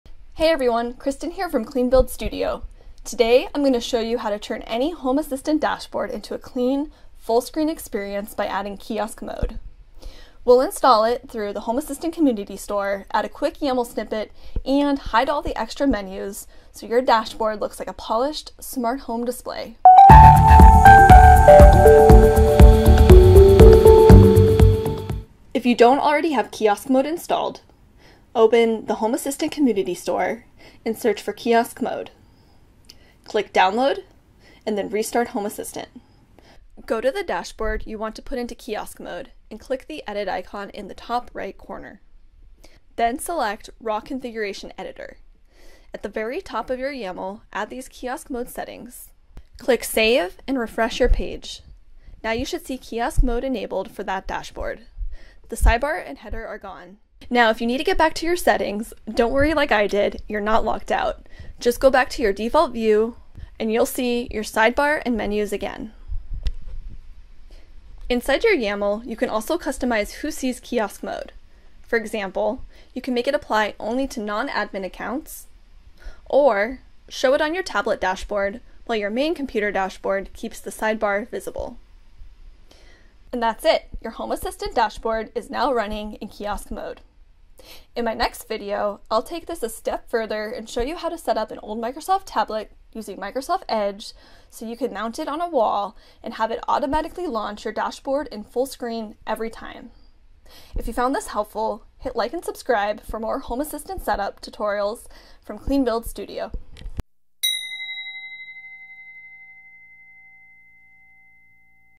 Home Assistant Kiosk Mode: Hide Sidebar & Header for a Clean Dashboard
Transform your Home Assistant dashboard into a clean, fullscreen display with Kiosk Mode.
In this quick tutorial, I’ll show you exactly how to hide the sidebar and header, install Kiosk Mode through HACS, and add a simple YAML snippet so your dashboard looks polished and distraction-free.

✅ What you’ll learn:
• How to install Kiosk Mode in Home Assistant using HACS
• Where to add the kiosk_mode: YAML settings
• How to hide the sidebar and top menu bar
• How to show the sidebar only for admin accounts

Perfect for wall-mounted tablets, touch panels, and anyone who wants a minimalist Home Assistant dashboard.